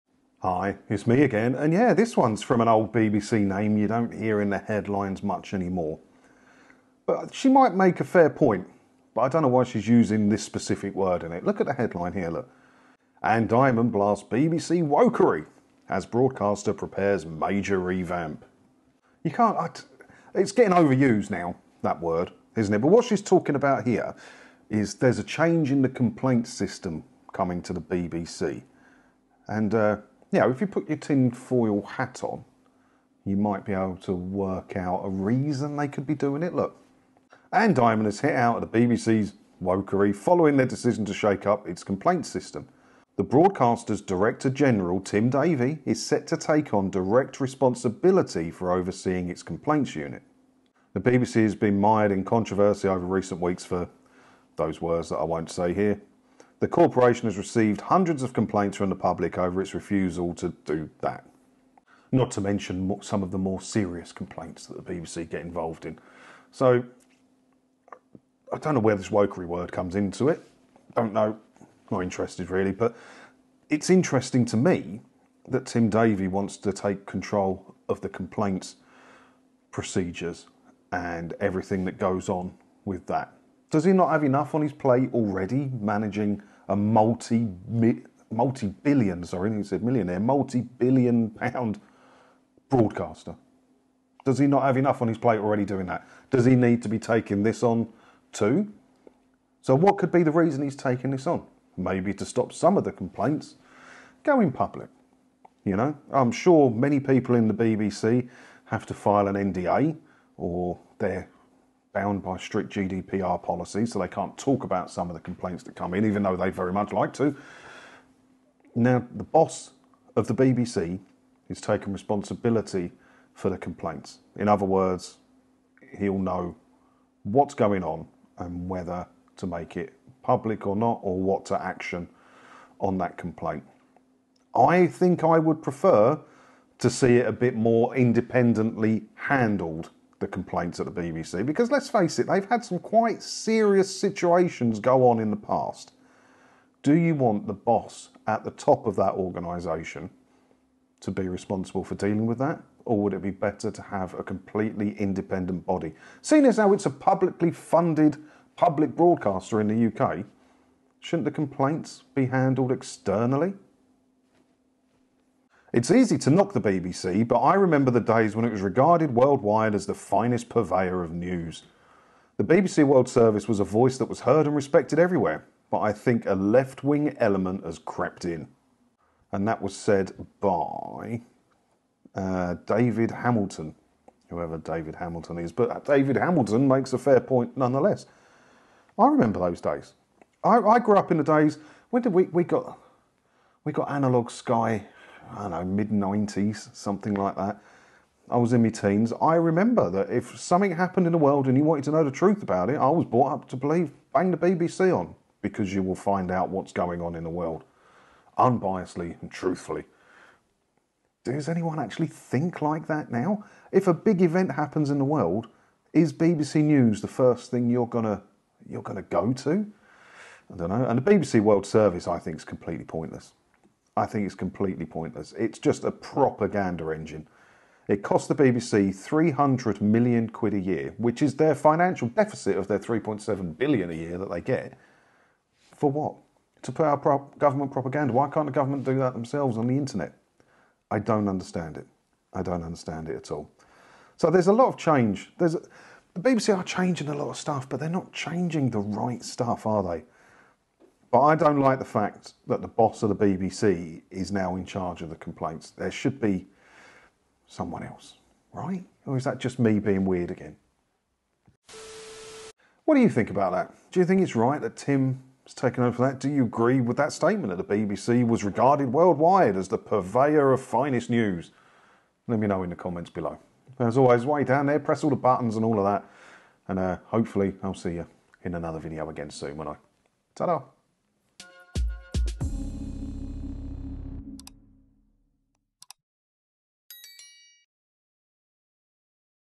Seem Suspicious To Me….
Why would the boss of the BBC want to be in charge of complaints? Does he not have enough to do? Shouldn’t it be managed externally?

You can do it here (for faster advice use the link above)

For TV Licence advice and help, take a look at the forum